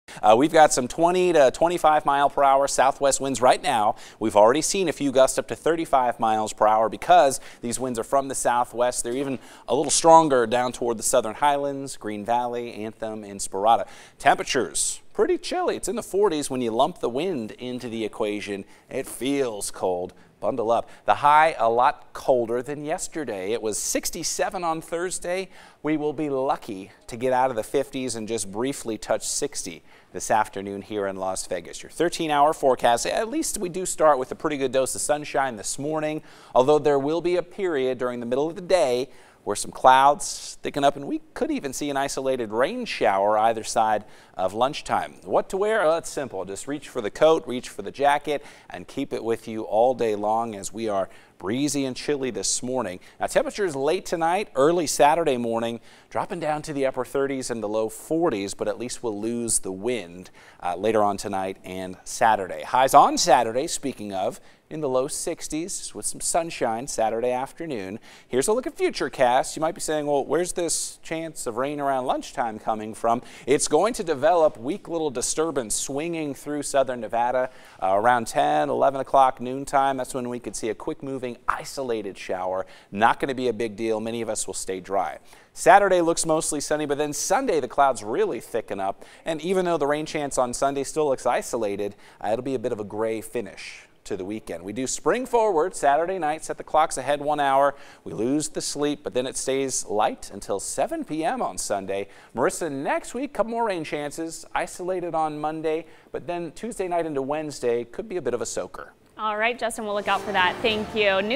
13 First Alert Las Vegas weather updated March 8 morning
Windy and chilly with a small rain chance.